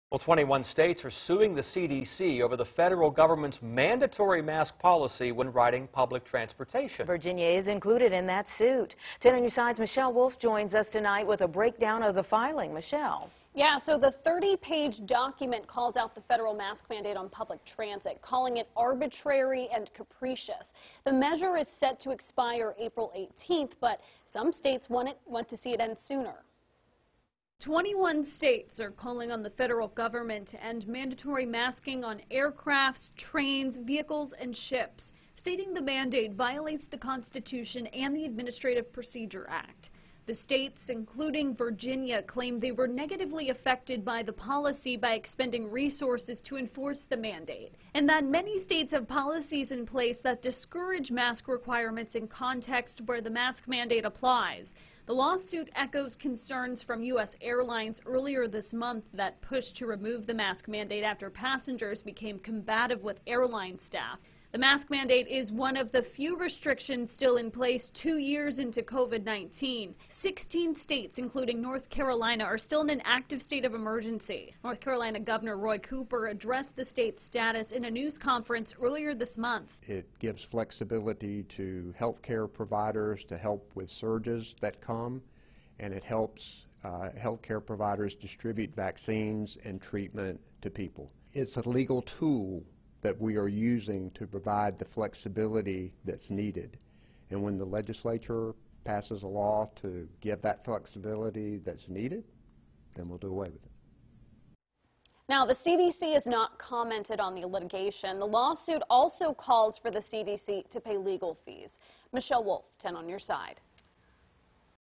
Virginia, other states challenge CDC transit mask rule
WAVY News 10's Michelle Wolf reports.